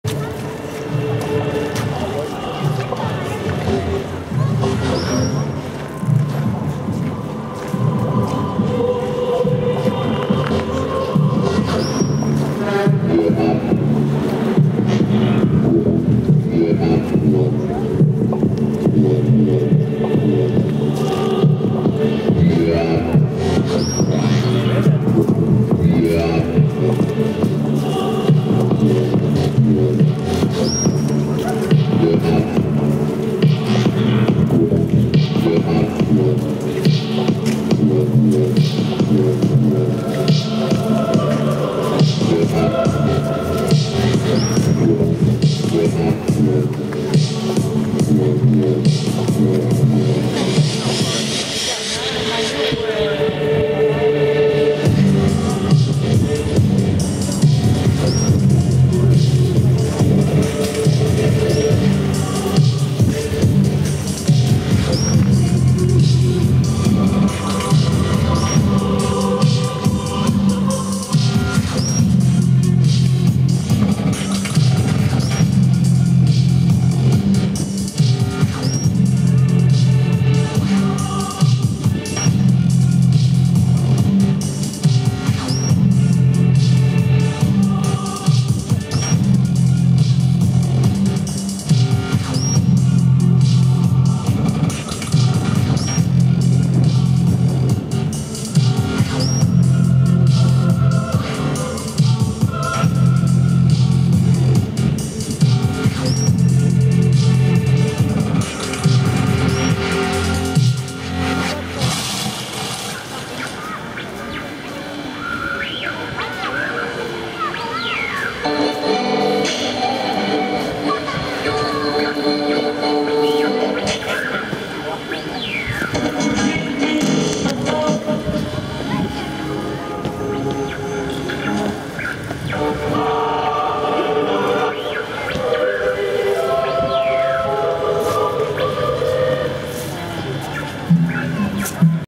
New Years Public Art Performance in Quebec City
Public art at a Cathedral in Quebec city